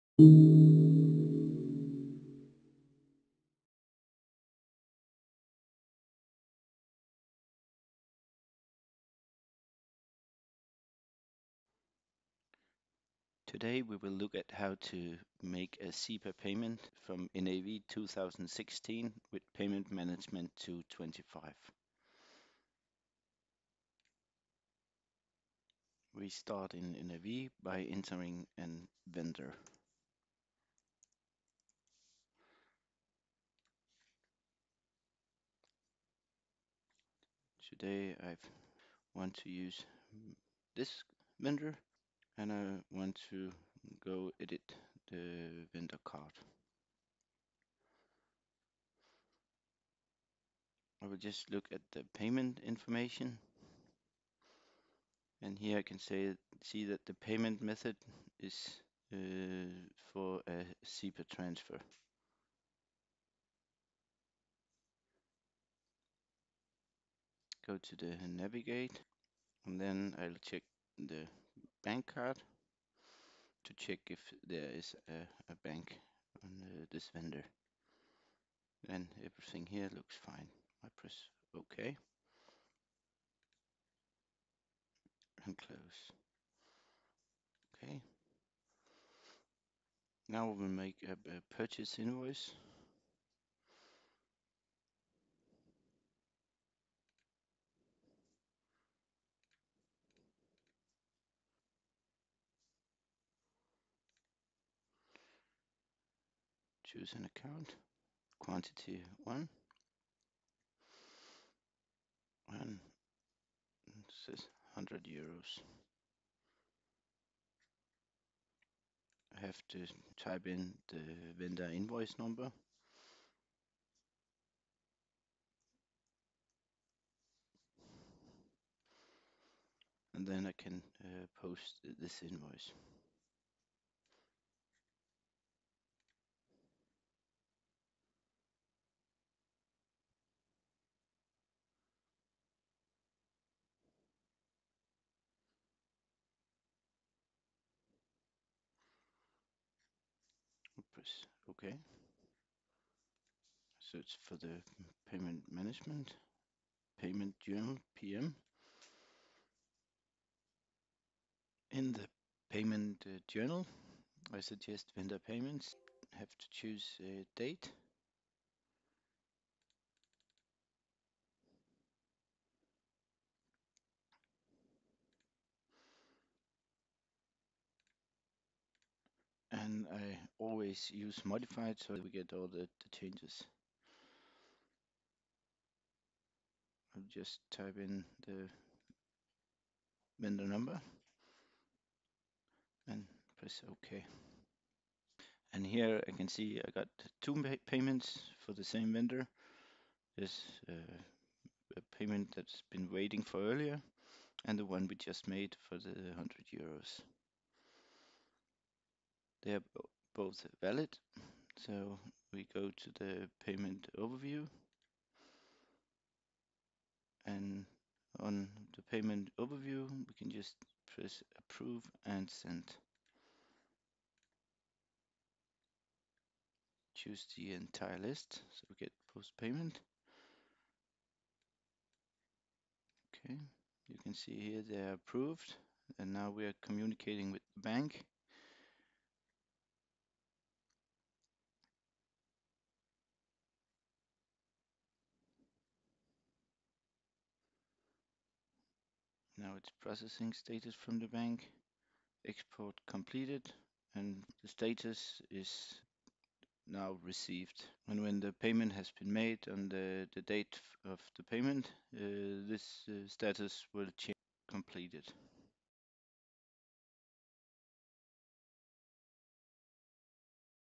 How to make a SEPA Payment in Continia Payment Management
By Product Specialist, Thomas Droob